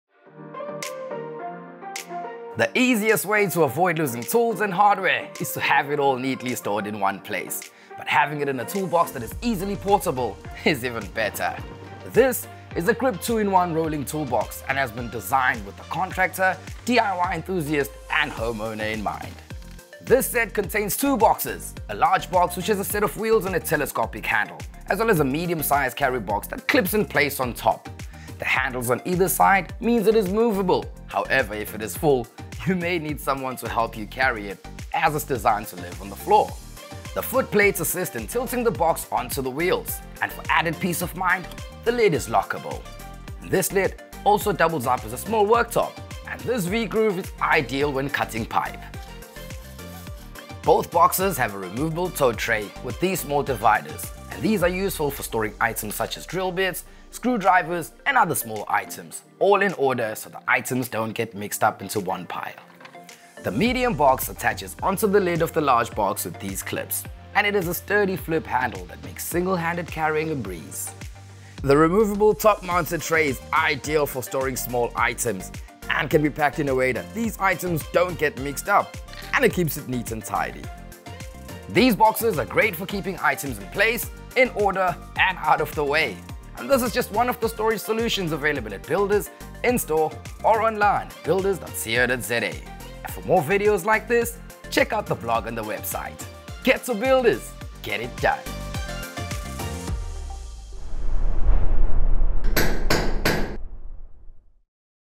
The Grip 2 in 1 Toolbox
A toolbox is a must for contractors and DIY enthusiasts who need to keep their tools organised and mobile. This Grip 2 in 1 rolling Toolbox has a large compartment on wheels that can accommodate most power tools and a smaller handheld storage compartment which clips to the larger compartment – ideal for most hand tools. XO speaks to all the features and benefits of this versatile combination.